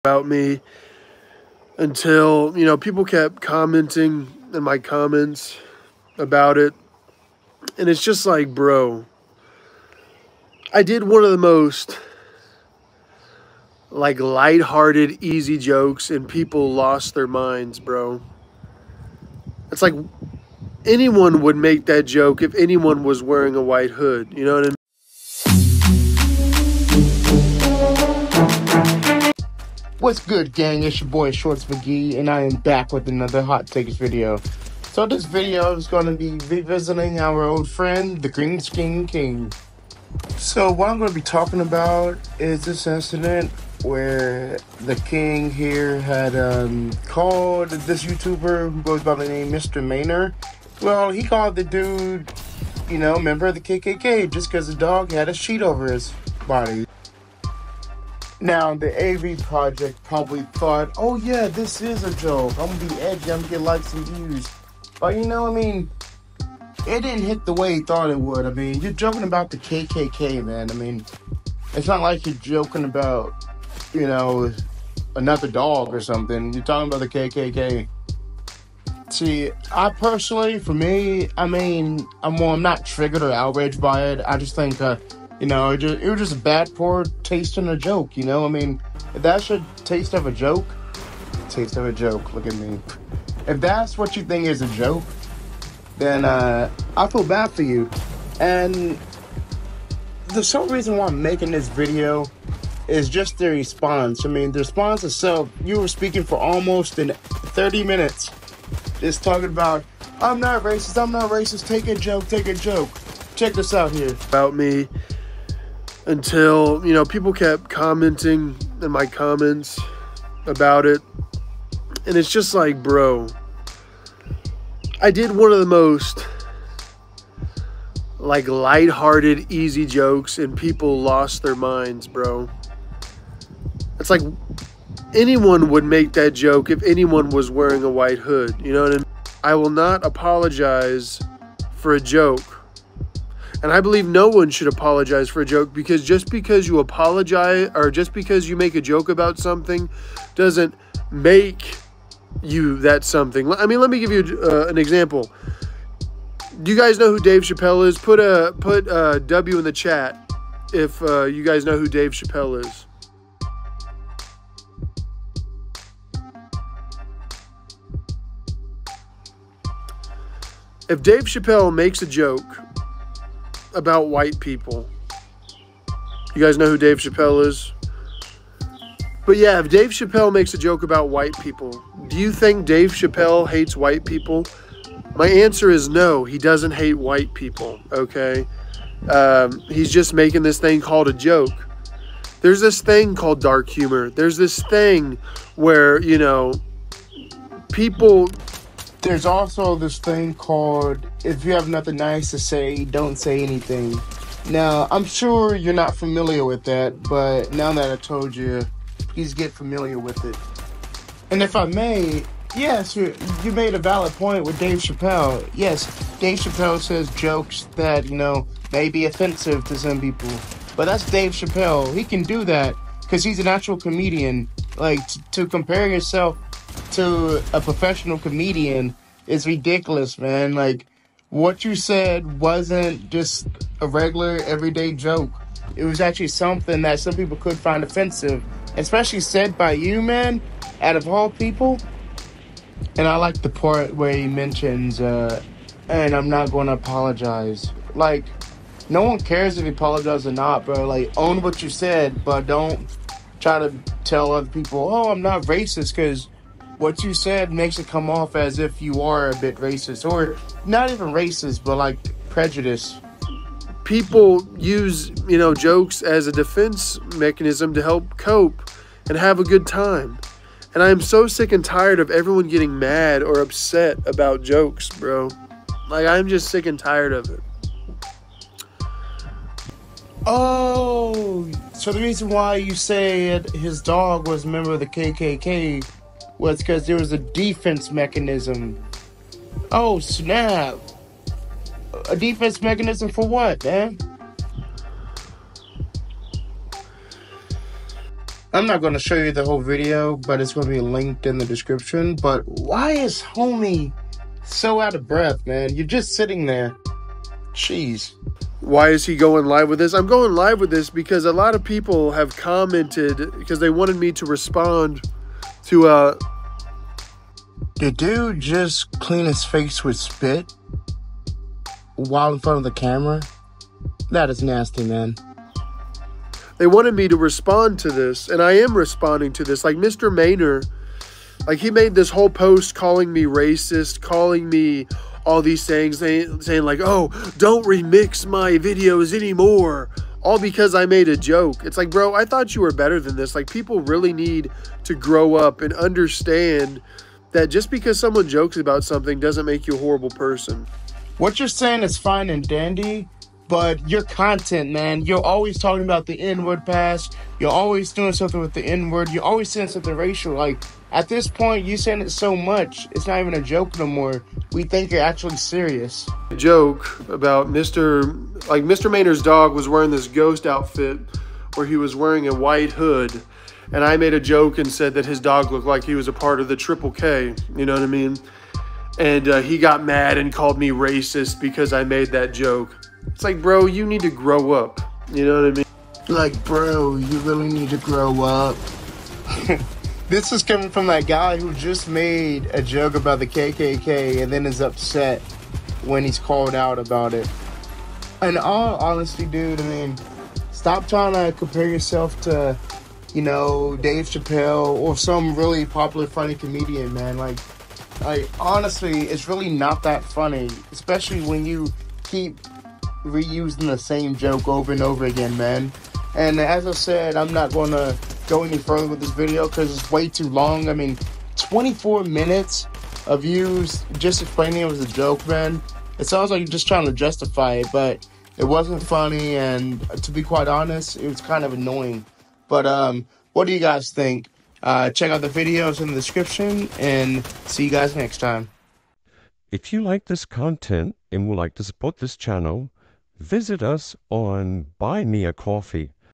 EP 10: This Creator is COPING
The YouTube Creator The AVProject came under fire in a now deleted video where he called another creator’s (Mister Mainer) dog a member of the Triple K (KKK), just for having a sheet over it’s head. He is posted a response video. Here’s my hot take.